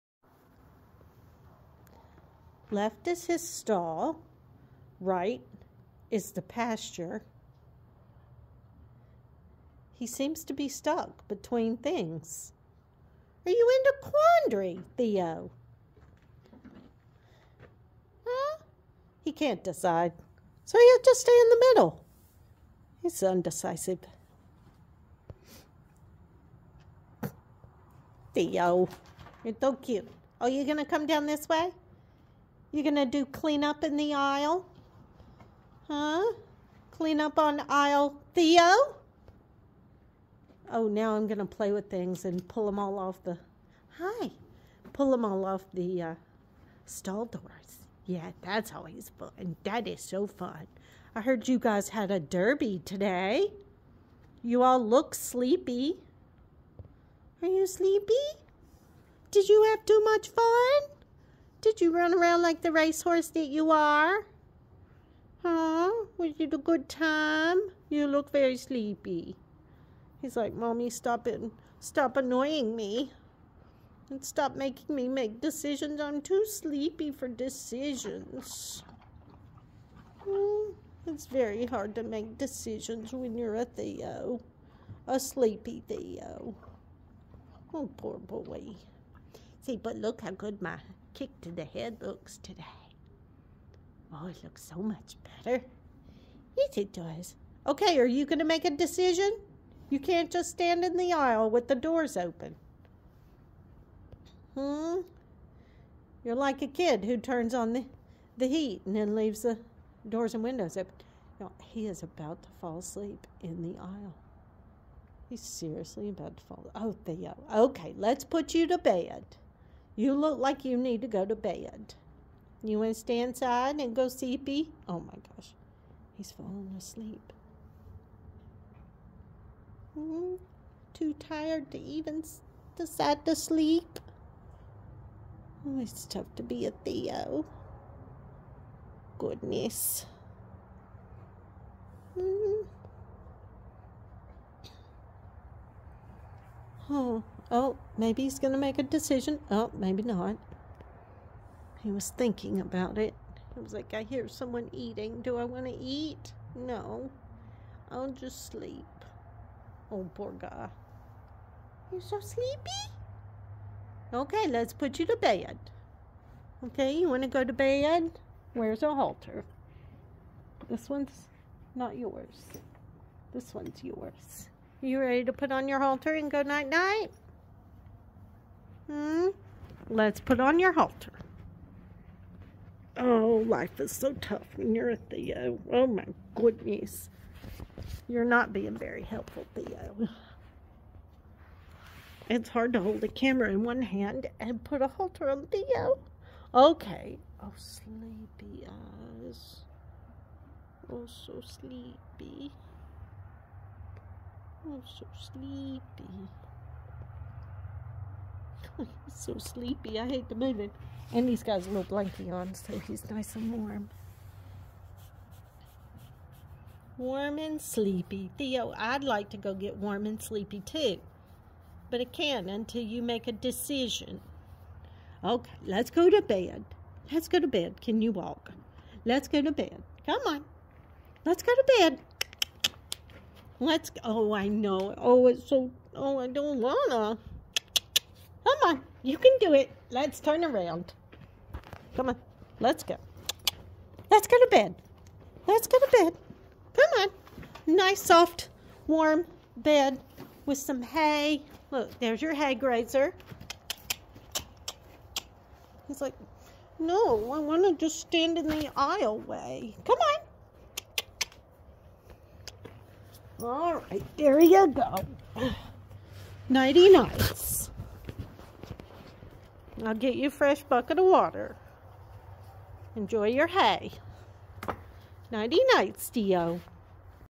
Ever been so tired you can't make a decision? 🤣
My ottb Theo finally got to go play in the big front field with more grass! I think they had fun and ate too much! 🤣